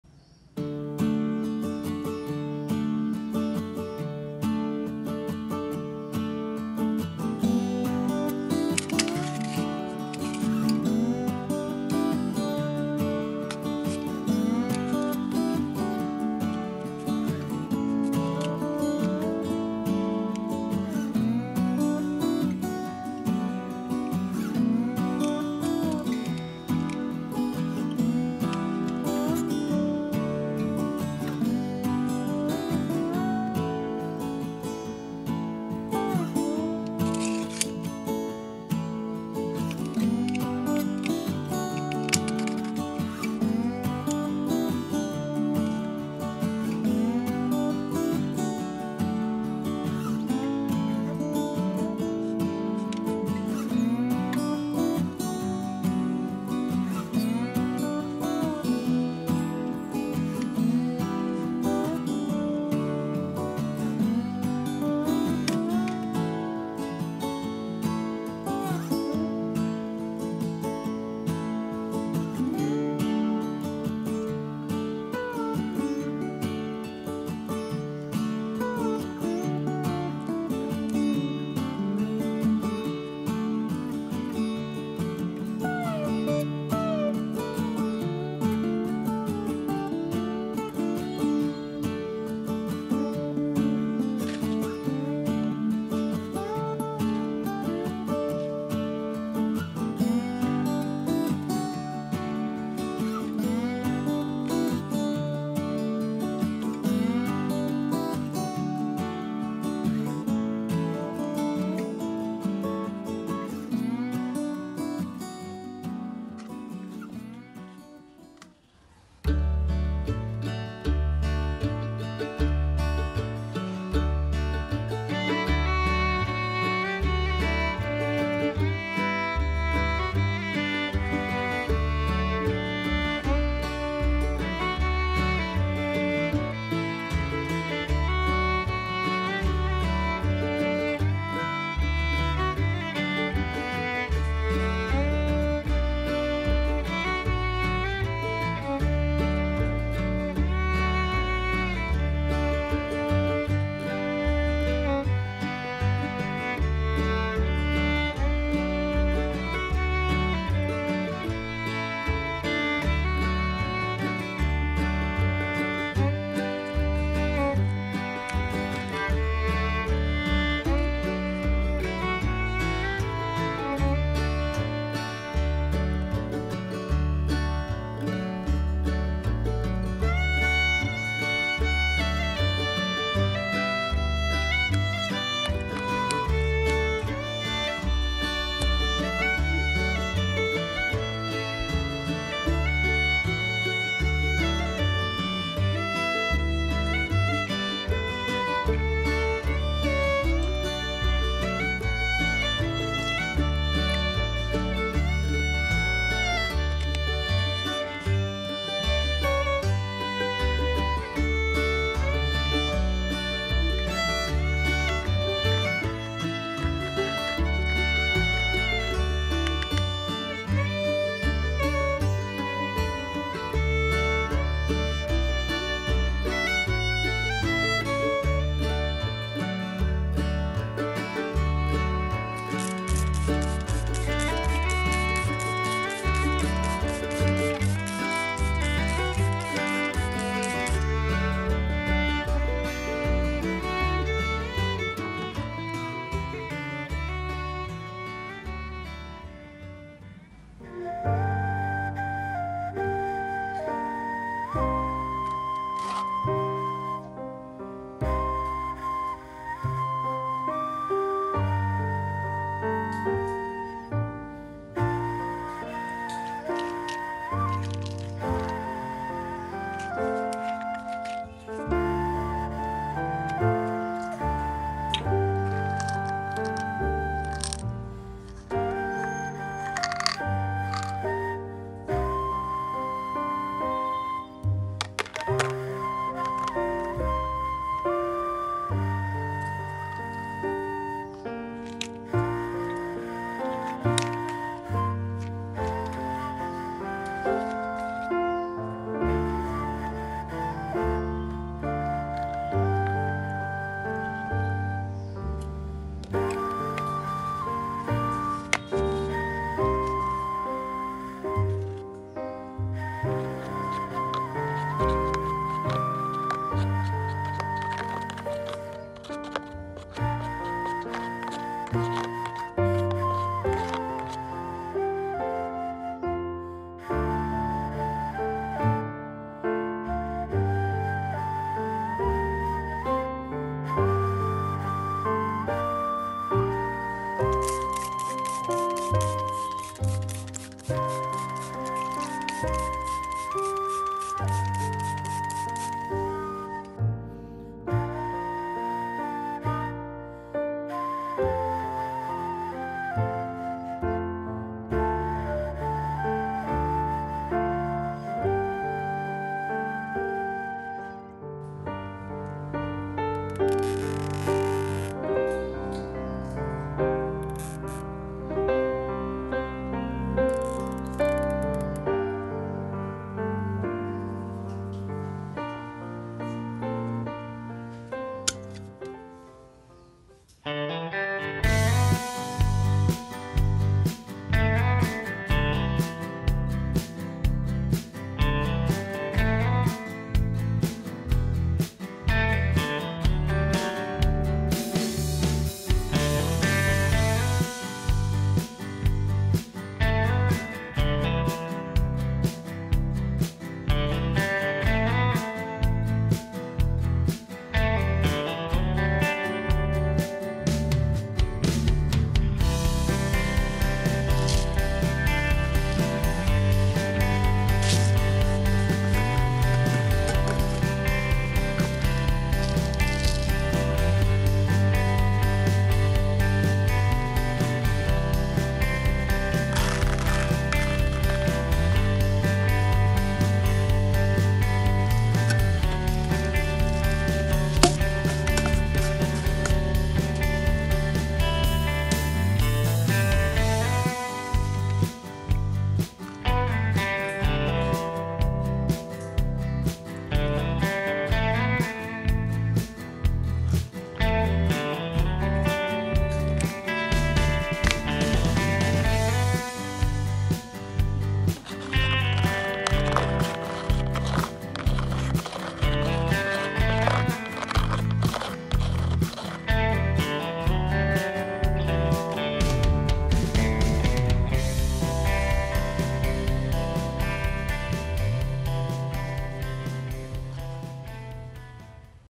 I was surprised to propagate cucumbers this way | Relax Garden
I was surprised to propagate cucumbers this way
Hello friends! Welcome to Relax Garden channel
In today's video I will show you how to propagate cucumbers that you don't know yet
Prepare the cucumbers and start following the instructions